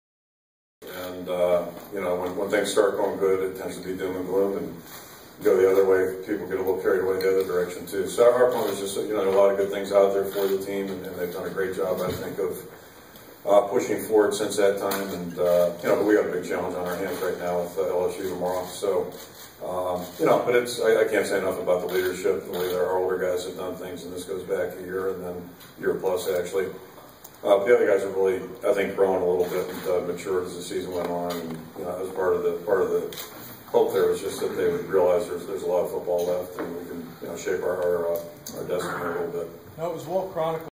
Iowa coach Kirk Ferentz
discusses how the Hawkeyes gave themselves a chance for a nine-win season following a conversation he had with the team after it won its sixth at Purdue